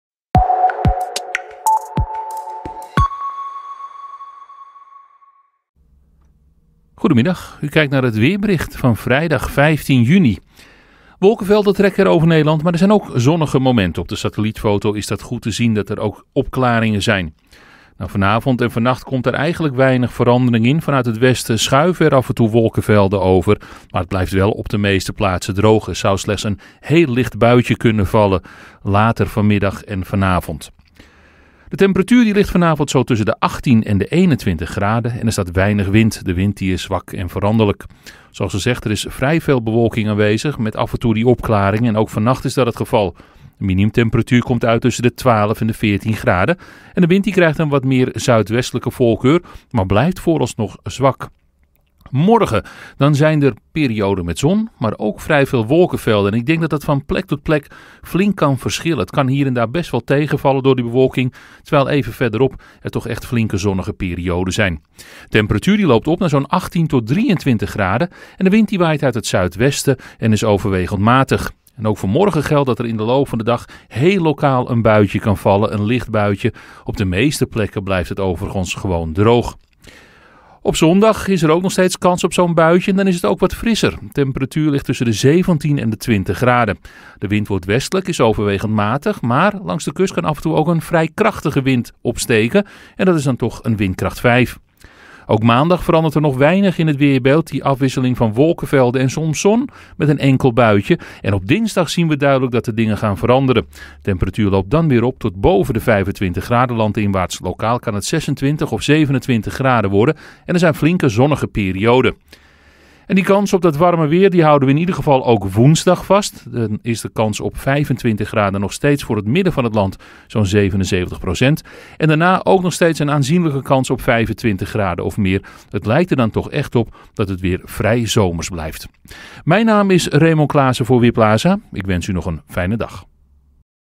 Weerbericht vrijdag 15 juni. Wolken en zon
Rustig weer is het de komende dagen met wolken, zon en een kleine kans op een bui.